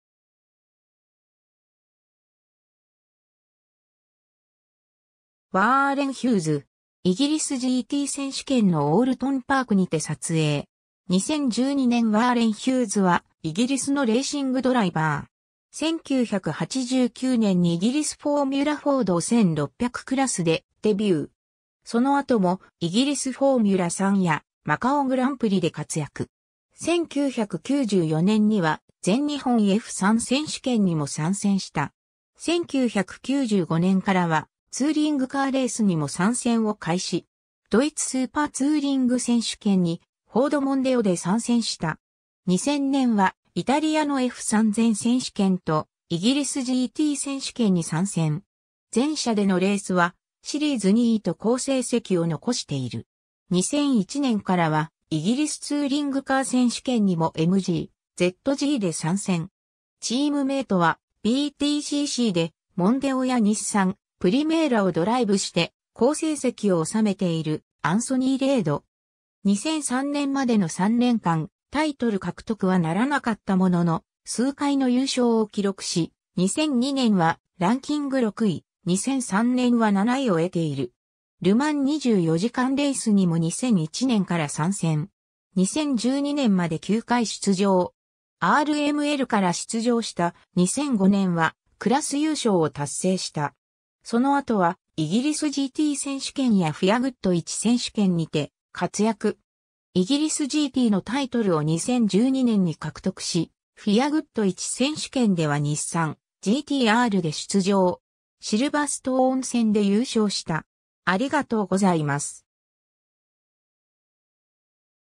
ワーレン・ヒューズ
イギリスGT選手権のオールトン・パークにて撮影。
2012年 ワーレン・ヒューズ（Warren Hughes、1969年1月19日 ｰ ）は、イギリスのレーシングドライバー。
 1989年にイギリスフォーミュラ・フォード1600クラスでデビュー。
その後もイギリスフォーミュラ3やマカオグランプリで活躍。
1994年には、全日本F3選手権にも参戦した。
1995年からは、ツーリングカーレースにも参戦を開始。
ドイツ・スーパーツーリング選手権(STWカップ)にフォード・モンデオで参戦した。
2000年はイタリアのF3000選手権とイギリスGT選手権に参戦。
前者でのレースは、シリーズ2位と好成績をのこしている。
2001年からは、イギリスツーリングカー選手権(BTCC)にもMG・ZGで参戦。
チームメイトは、BTCCでモンデオや日産・プリメーラをドライブして好成績を収めている、アンソニー・レイド。
2003年までの3年間、タイトル獲得はならなかったものの、数回の優勝を記録し、2002年はランキング6位、2003年は7位を得ている。
 ル・マン24時間レースにも2001年から参戦。
2012年まで9回出場。
RMLから出場した、2005年はクラス優勝を達成した。
その後はイギリスGT選手権やFIAGT1選手権にて活躍。
イギリスGTのタイトルを2012年に獲得し、FIAGT1選手権では日産・GT-Rで出場。
シルバーストーン戦で優勝した。